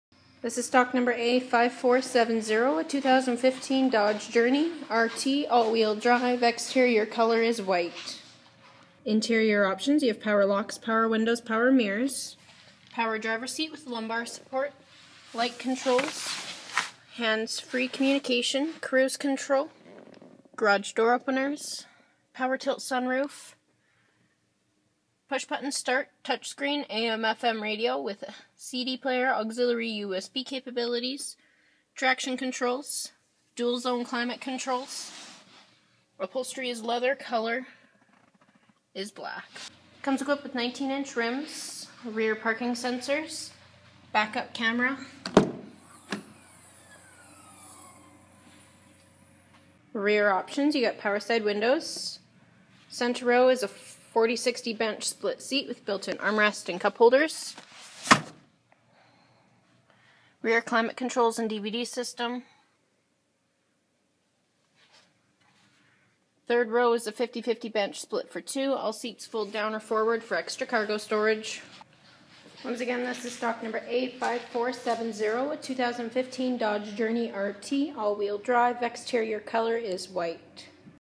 2015 Dodge Journey AWD 4dr R/T
This is a 2015 Dodge Journey AWD 4dr R/T with 6-Speed A/T transmission White[PW3,White] color and Black interior color.  This video is recorded and uploaded by cDemo Mobile Inspector